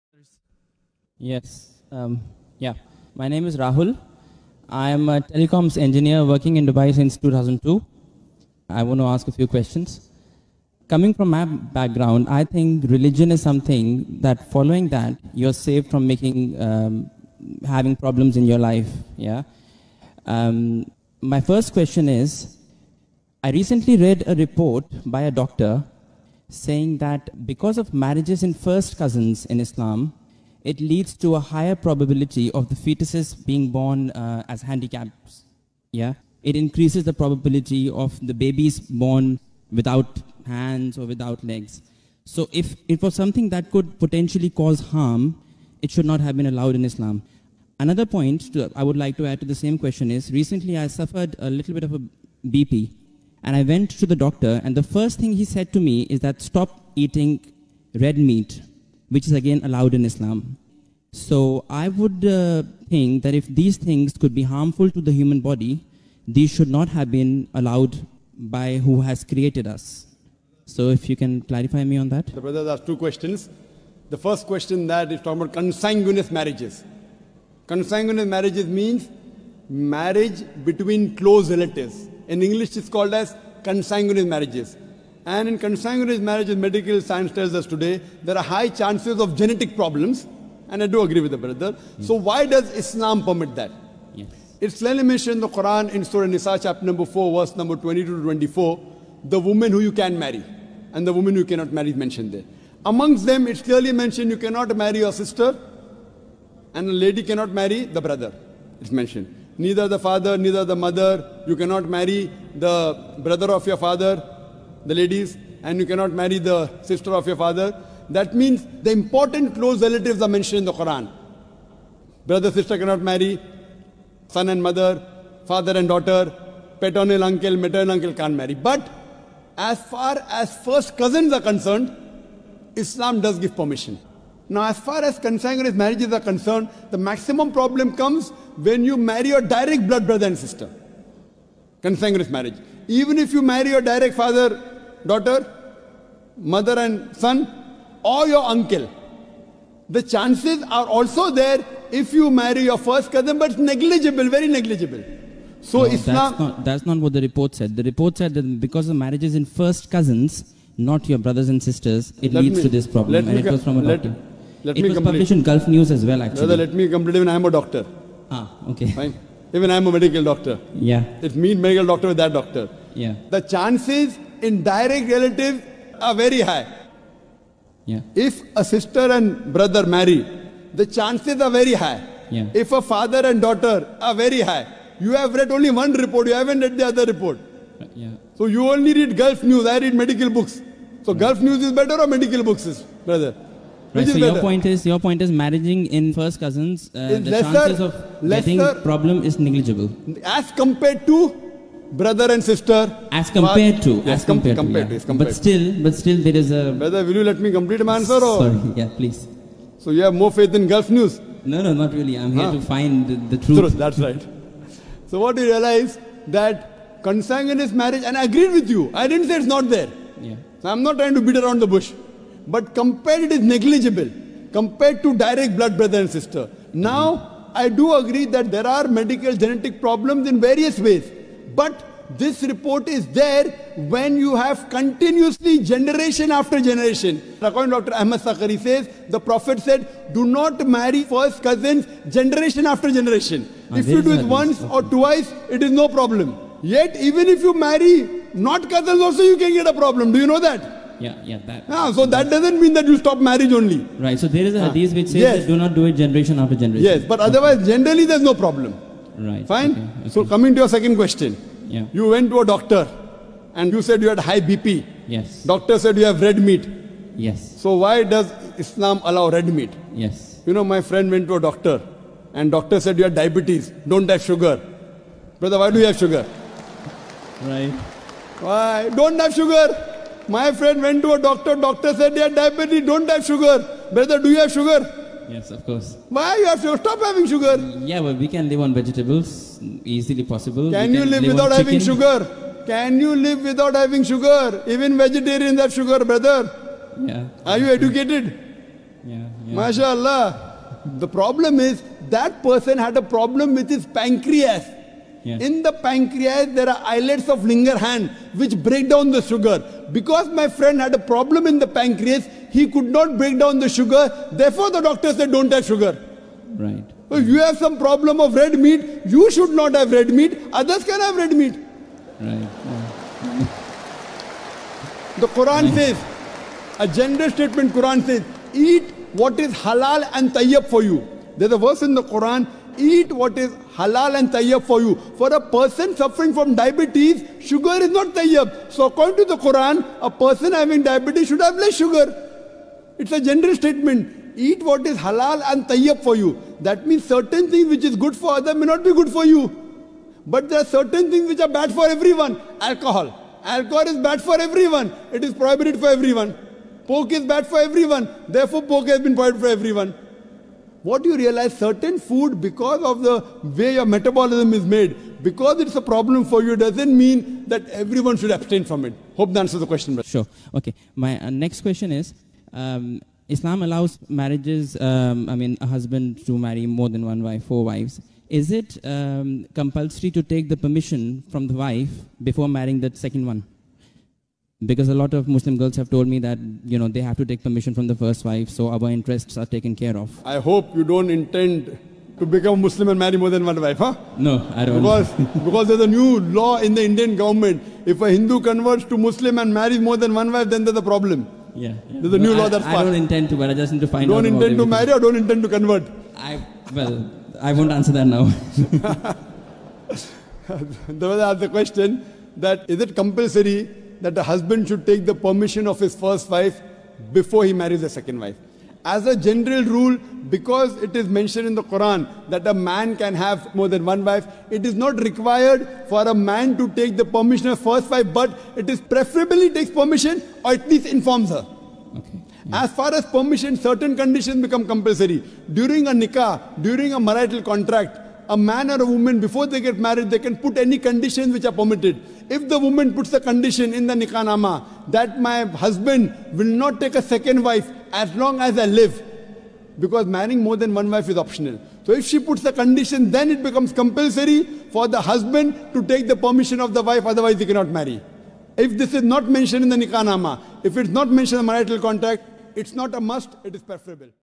Why nonsecular marriages and food that harm body allowed in Islam by dr zakir naik
A man ask very good question that why nonsecular marriages and food that harm body allowed in Islam dr gives a good reply that it is clearly and by name mention in Quraan  that which relative should not marriage and answer the second question that the things which permit doctors for a person it is harmful only for that person not for all